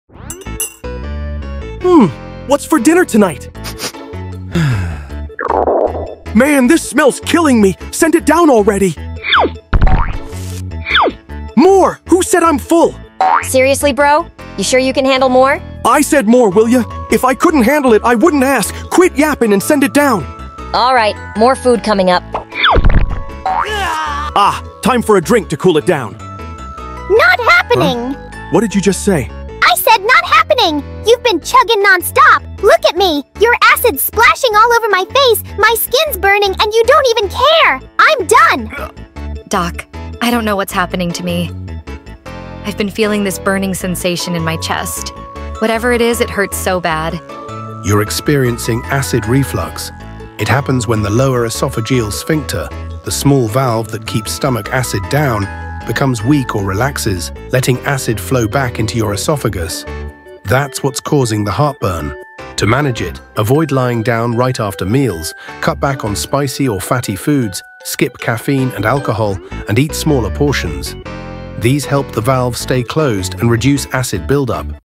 What actually causes acid reflux?
Acid reflux happens when the valve that keeps stomach acid in place becomes weak and relaxes.
When this happens, acid moves upward into the esophagus, causing chest burning, sour taste, and discomfort.
This often happens after overeating, eating late at night, spicy or fatty meals, or drinking alcohol and caffeine.
Understanding how it works can help you avoid it.